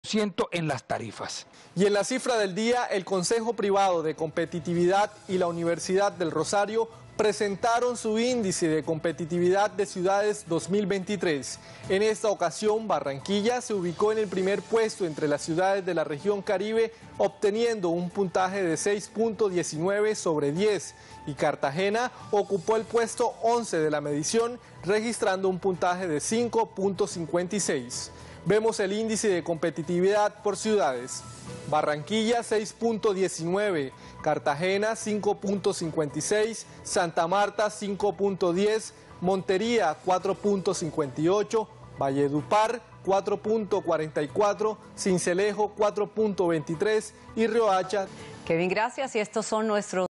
Barranquilla, la ciudad más competitiva de la región Caribe
El índice de Competitividad por Ciudades de la Universidad del Rosario y el Consejo Privado de Competitividad reveló el desempeño de las ciudades capitales más y menos competitivas del país.